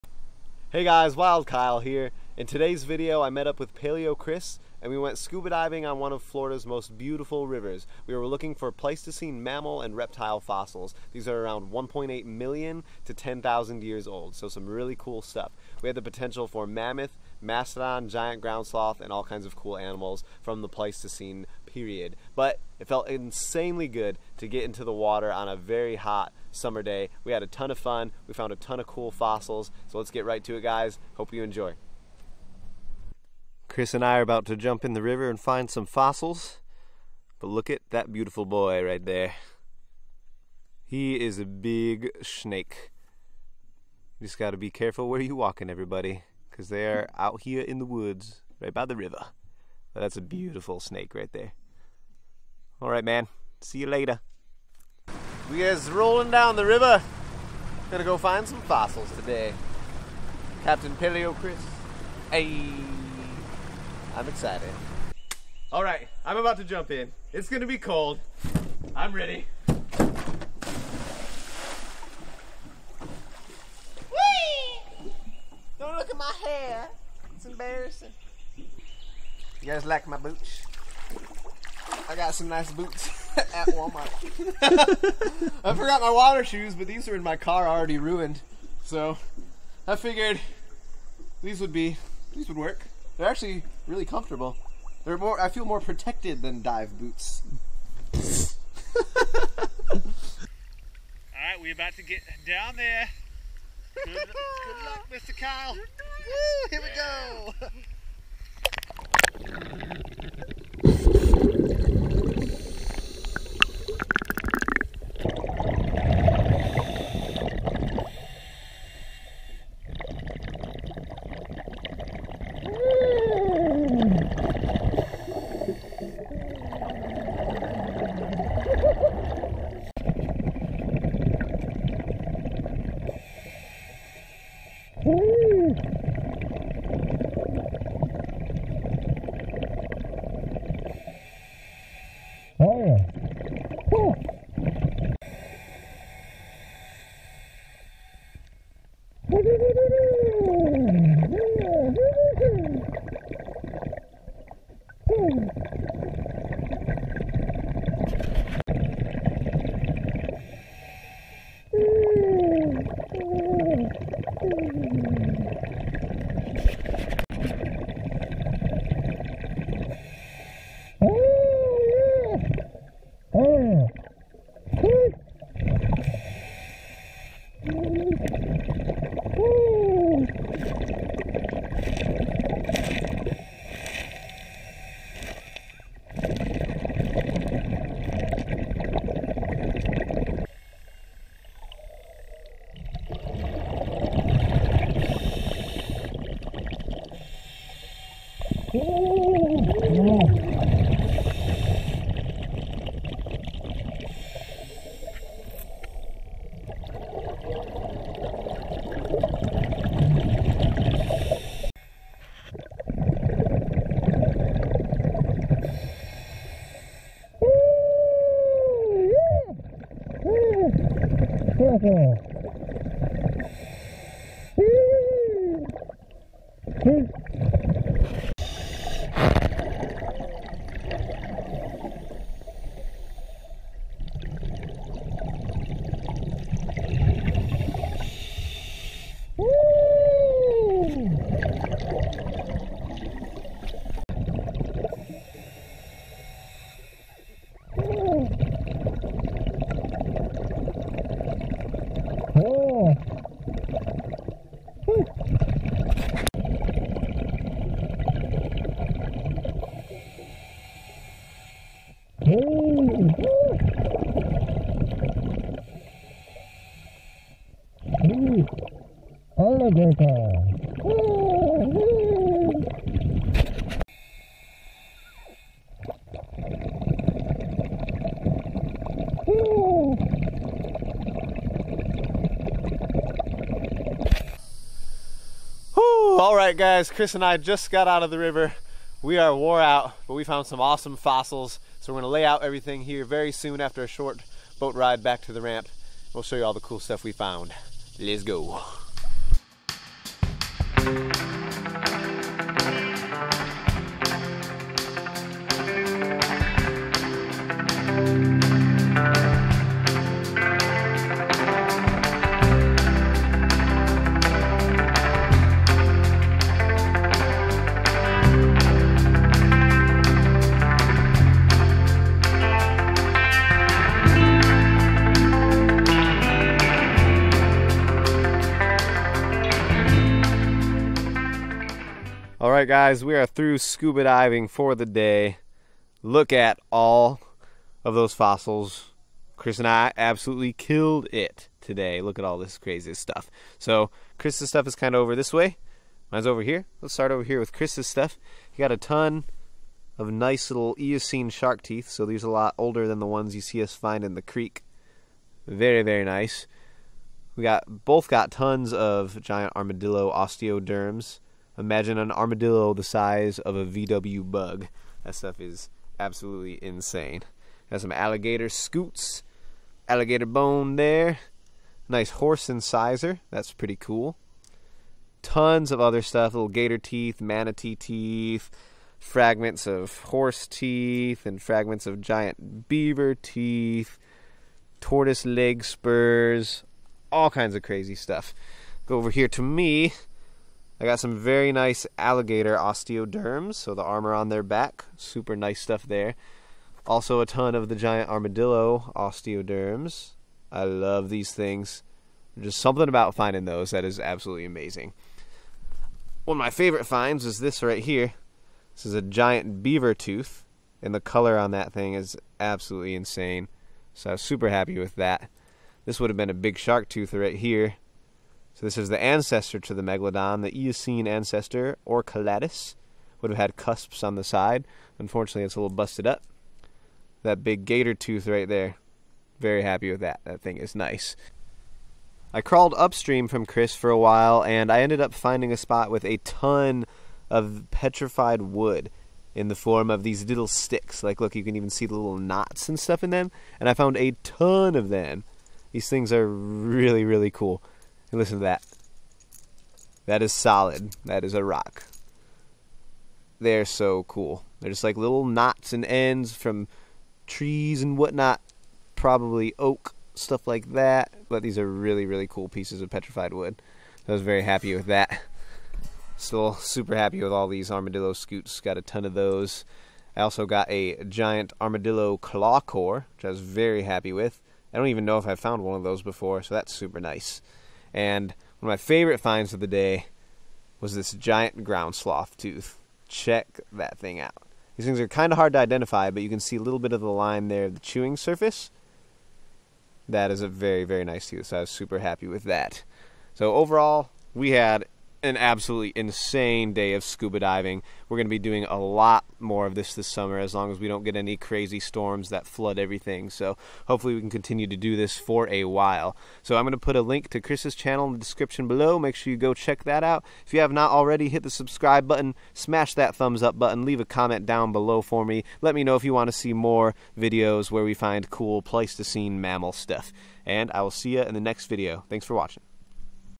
I Found a Giant Ground Sloth Tooth SCUBA Diving in a Florida River! - Finding Ice Age Fossils!
It is HOT, so getting to spend some time underwater and do some SCUBA Diving felt amazing! On top of that, we were looking for Ice Age fossils in a beautiful Florida River. It was an incredible day. We ended up finding fossils from Giant Ground Sloth, Giant Armadillo, Giant Tortoise, Horse, Shark, and all sorts of other amazing fossils!